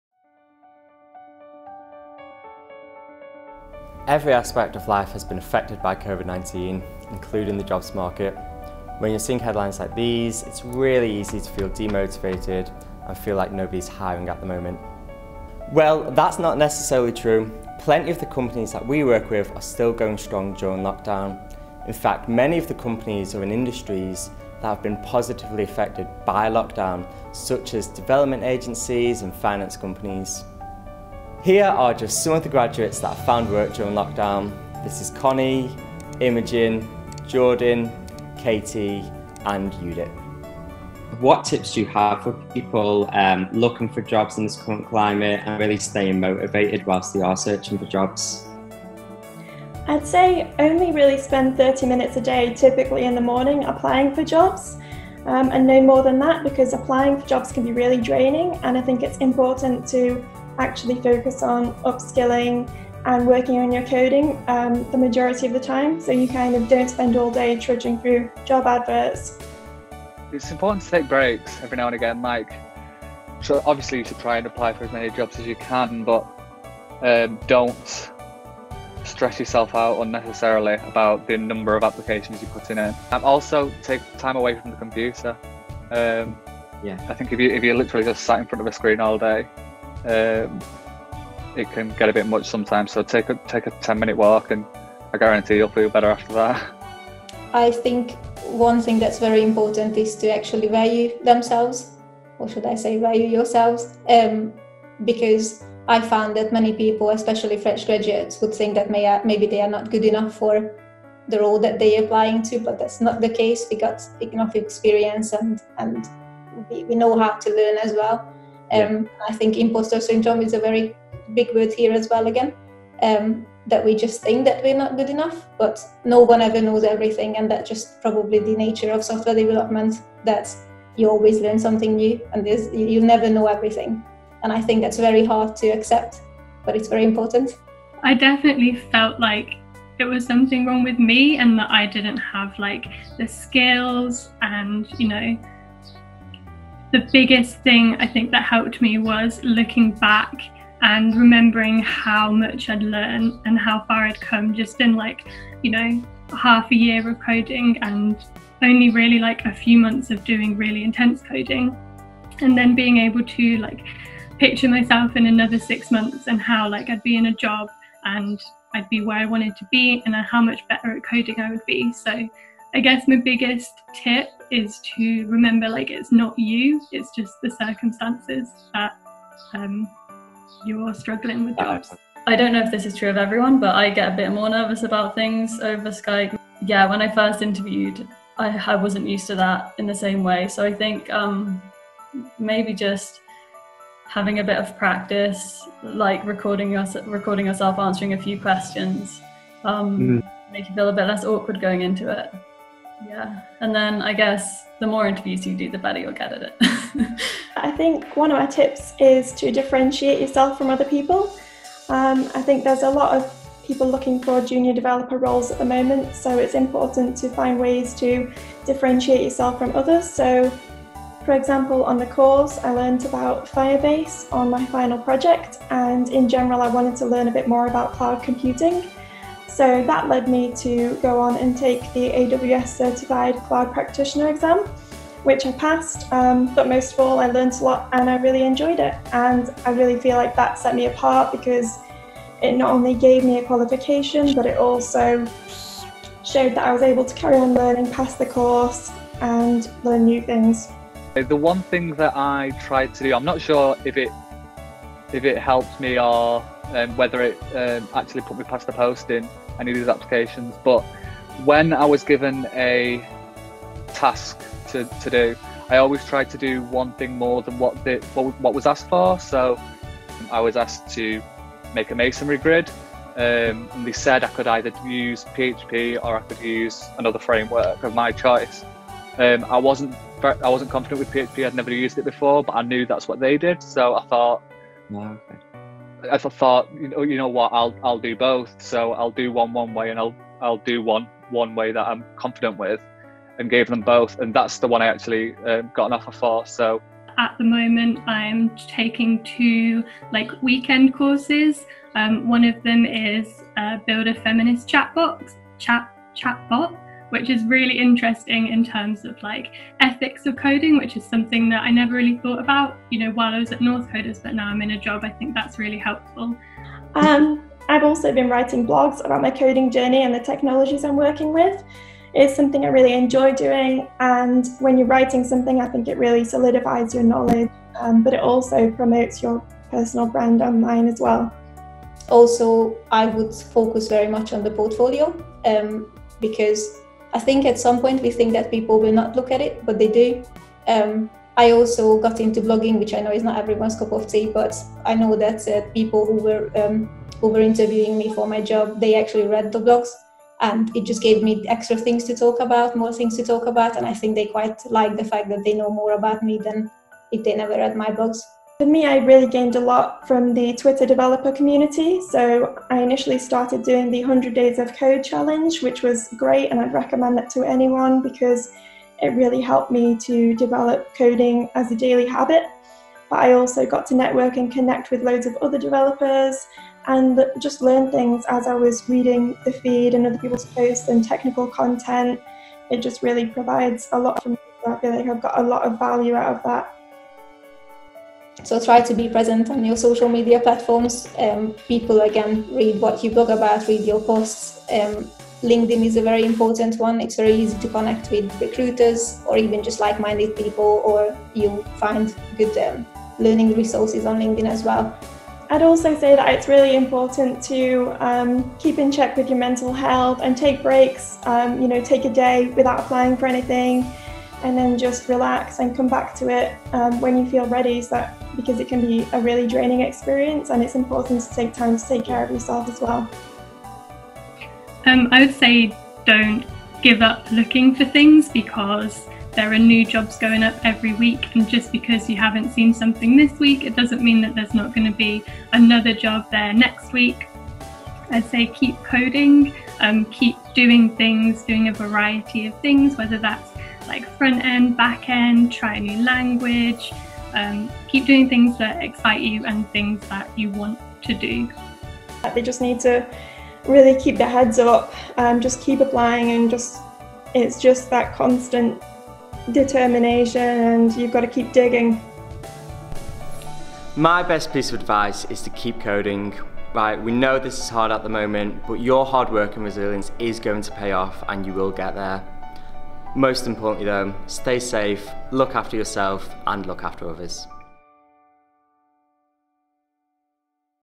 Top tips on finding work - From Northcoders Graduates who found work during lockdown.
We spoke to five Northcoders graduates who found their jobs during lockdown about their tips for staying motivated whilst job hunting.